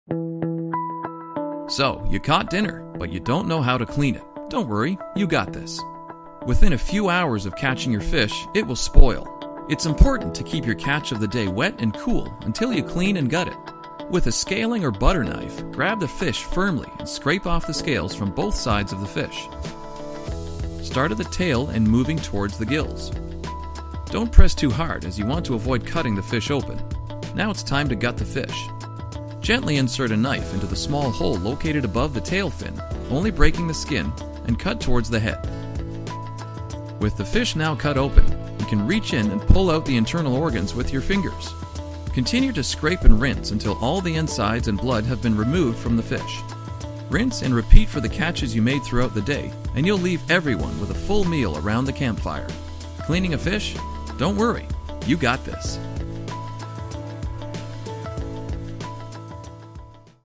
How to Clean a Fish (5 Steps)
So you caught dinner… but you don’t know how to clean it. Don’t worry you got this.